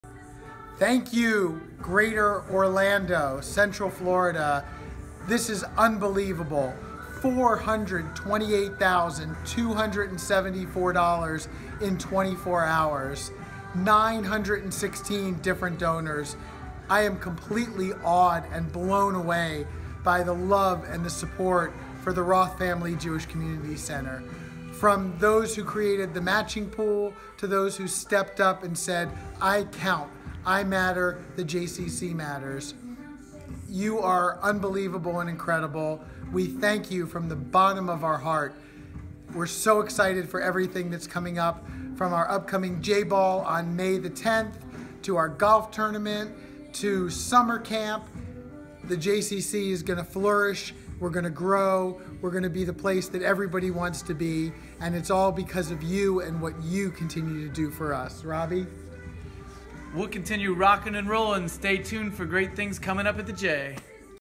We raised $428,274 in 24 hours!
CEO Keith Dvorchik and Executive Director Robby Etzkin thank you for your support!